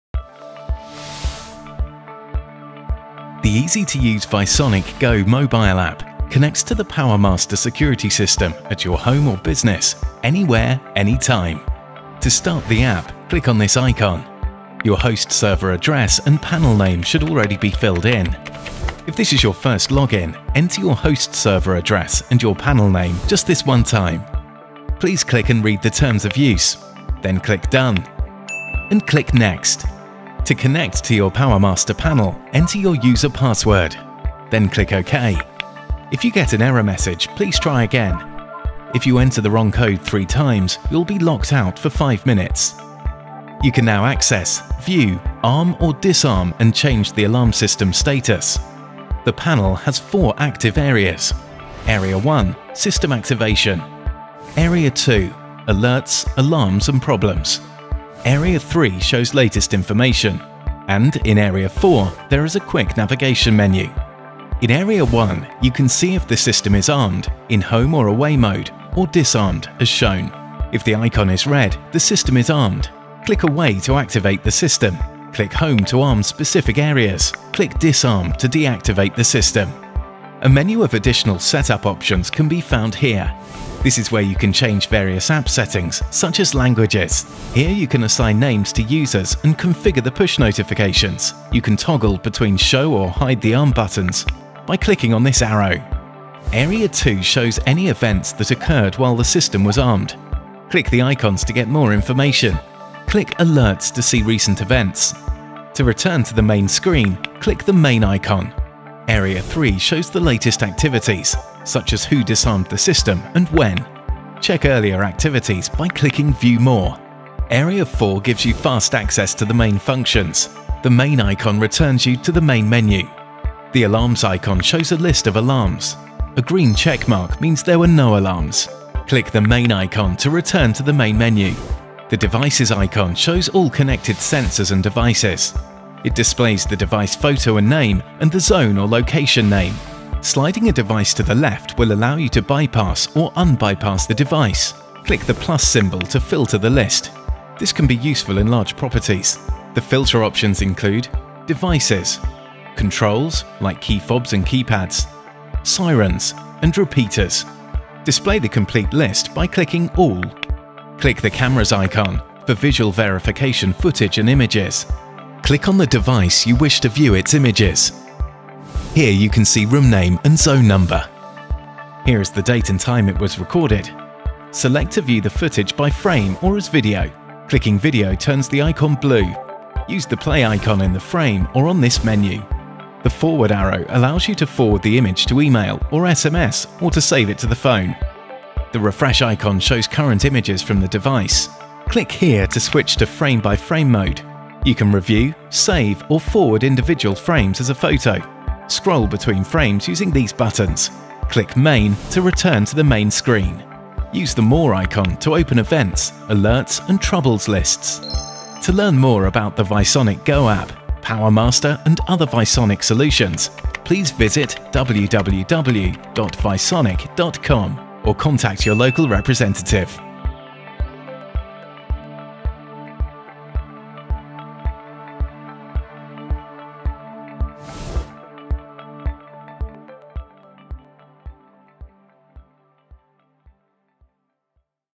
How to Use the VisonicGO Mobile App
Video tutorial and step by step guide on how to use the VisonicGO Mobile App for Visonic Powermaster range of Alarms. This app works with Powermaster 10, Powermaster 30 and Powermaster 33 alarms from Visonic